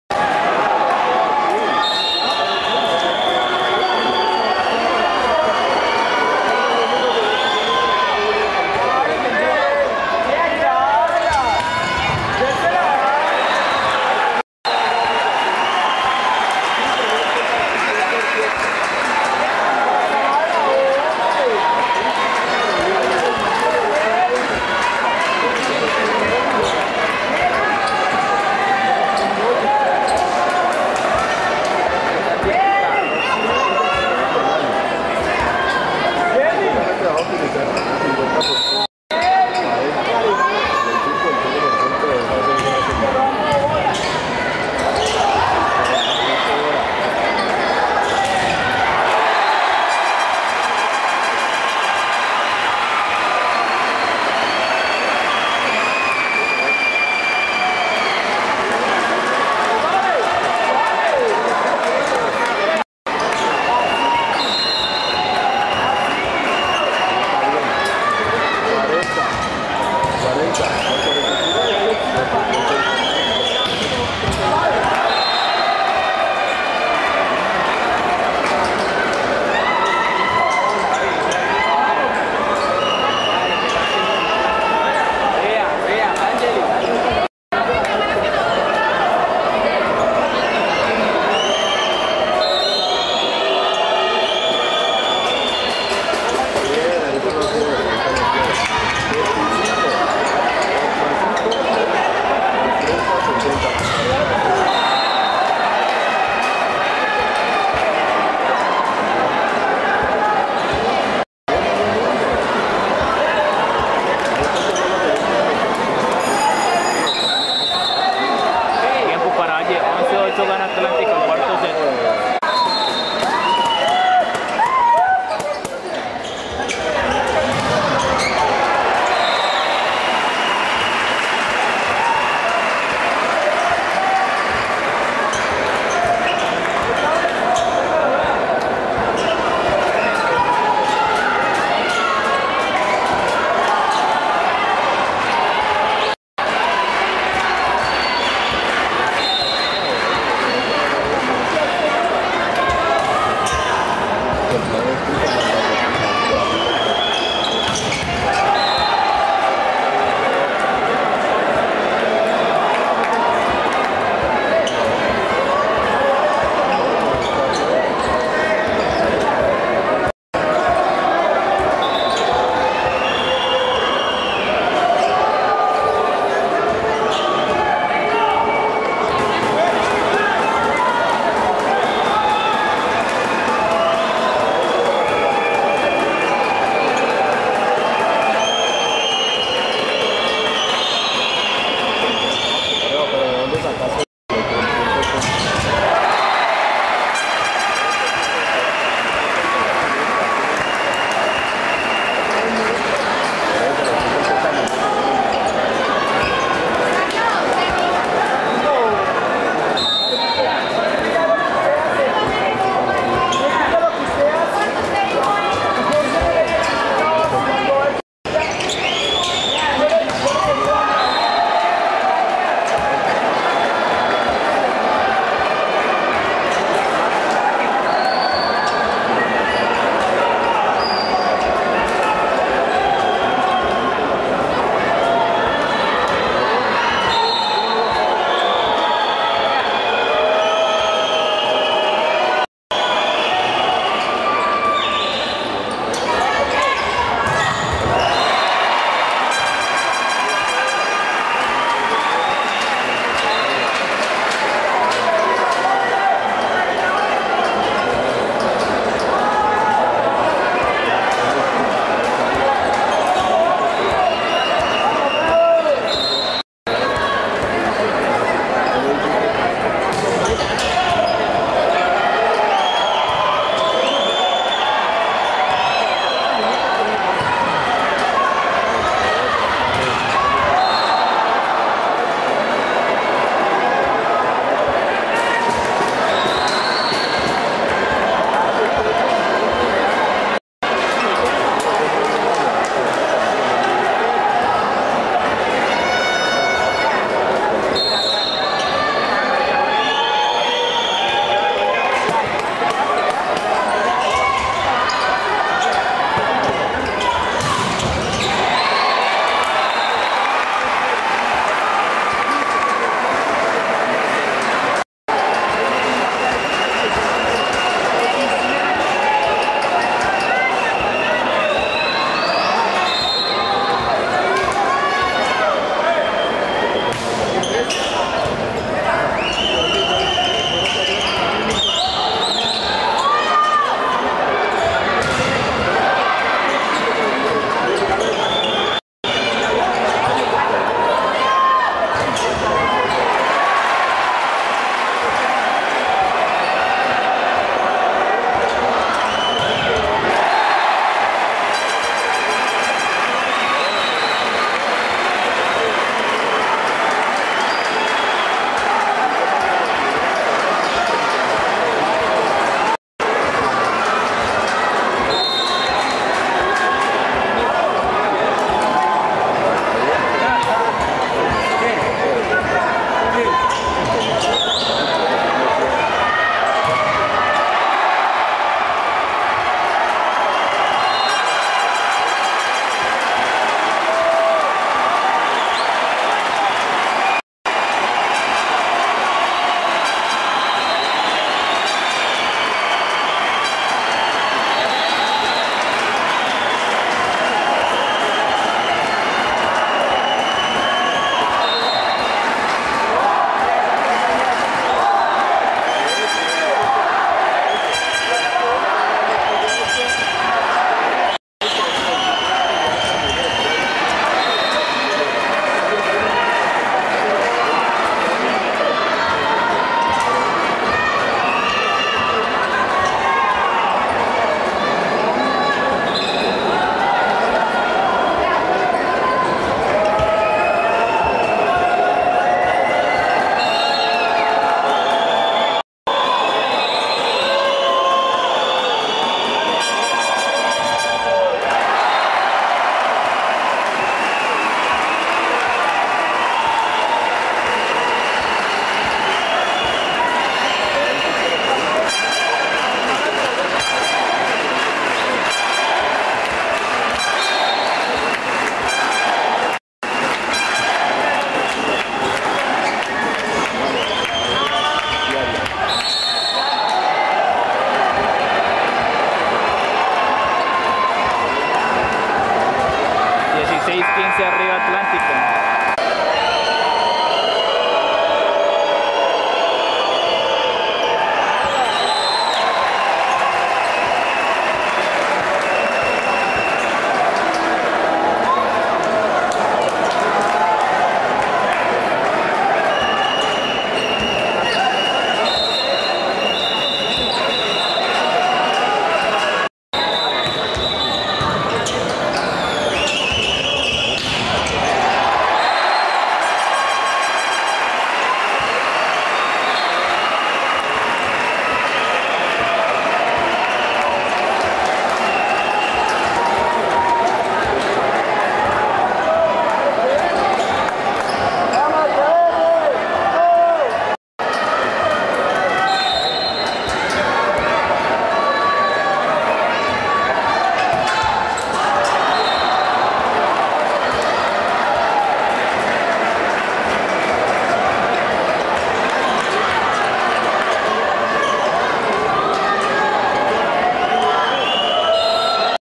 Francisco E.R. Volleyball Colombia Part 2
This video contains the last set of the national semifinal, in the Colombian National Games, Cali, Colombia. I am the Valle`s setter (white jersey).
We won the match 3 -1 against Atlantico.